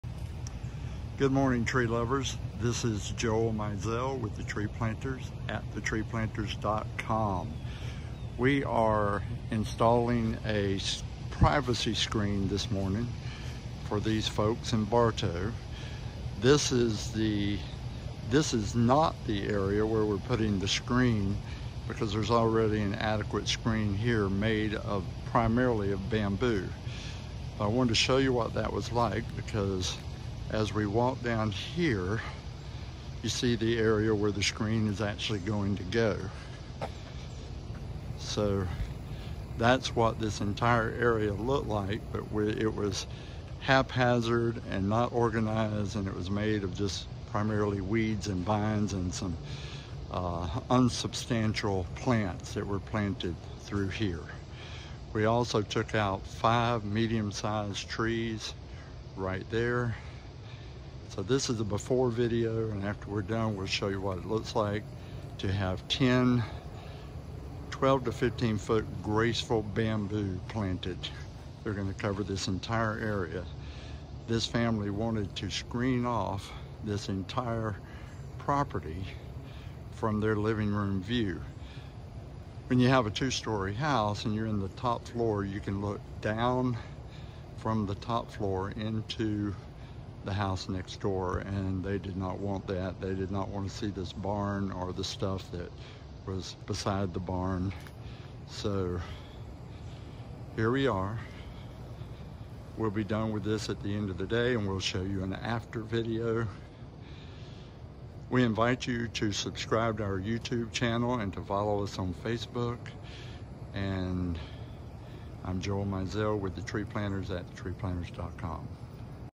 Graceful Bamboo Privacy Before Vid/The Tree Planters/Planted and Guaranteed/Fifty Years Experience
The Tree Planters Presents Our Graceful Bamboo.
A vigorous variety with a dense clumping habit of growth, making it perfectly suited for fast privacy screens; new culms are deep green, then mature to a yellow-green; ideal for containers and well lit indoor environments.
Graceful Bamboo is a dense herbaceous evergreen perennial with a rigidly upright and towering form. It brings an extremely fine and delicate texture to the garden composition and should be used to full effect.

This is a relatively low maintenance plant, and is best cleaned up in early spring before it resumes active growth for the season. It has no significant negative characteristics.

Graceful Bamboo is recommended for the following landscape applications;

Mass Planting
Hedges/Screening
Windbreaks and Shelterbelts
Naturalizing And Woodland Gardens
Container Planting